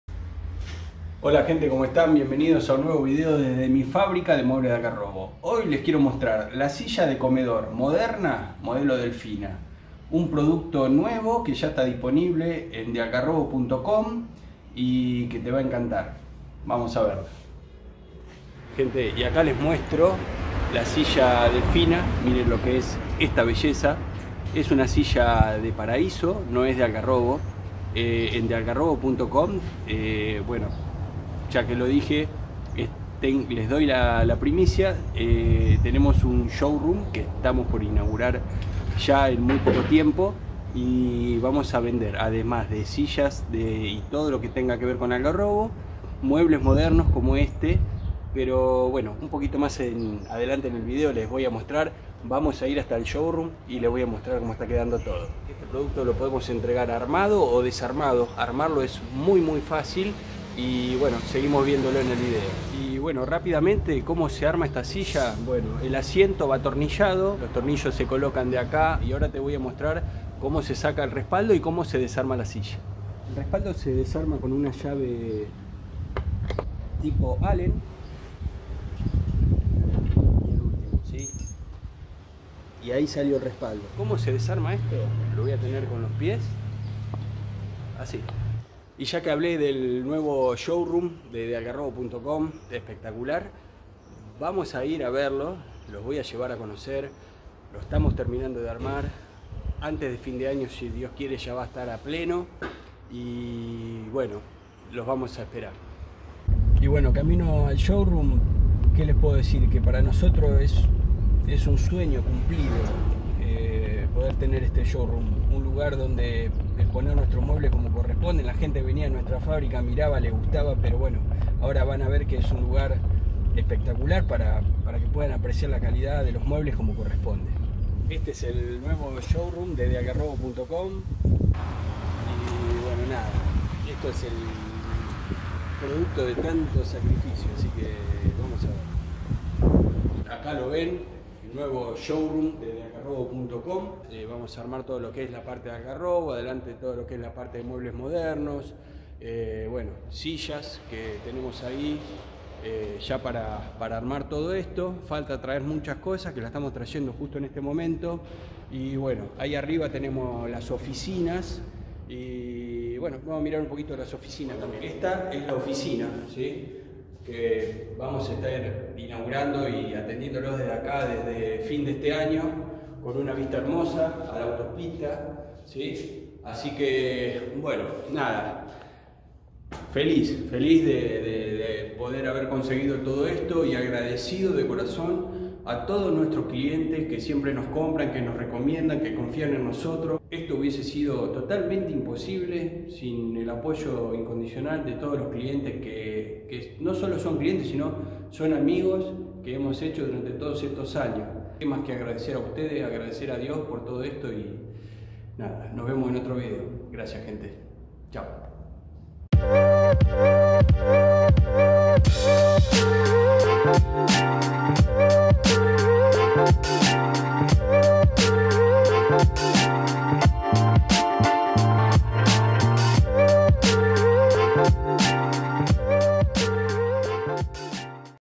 Silla de Comedor Moderna | dealgarrobo.com
Le mostramos nuestra silla de comedor moderna modelo Delfina
Para ver el precio actualizado y mas detalles de este producto ingrese a:

Silla Moderna Tapizada
Con su espectacular diseño nórdico la silla Delfina esta marcando tendencia. Se trata de una silla fabricada en madera maciza de paraíso con sentadera y respaldo tapizado lo que la hace una silla muy cómoda.

Más Información
Esta silla está fabricada con máquinas de última generación y lustrada en cabina de pintura de avanzada. Su asiento y respaldo ergonómico proporcionan un excelente soporte a la espalda siendo una de las sillas más cómodas de nuestro catálogo.

Sillas Modernas
Estamos incorporando a nuestro catálogo una nueva línea de sillas modernas de madera tapizadas con diseños innovadores como ésta y otras que publicaremos en breve.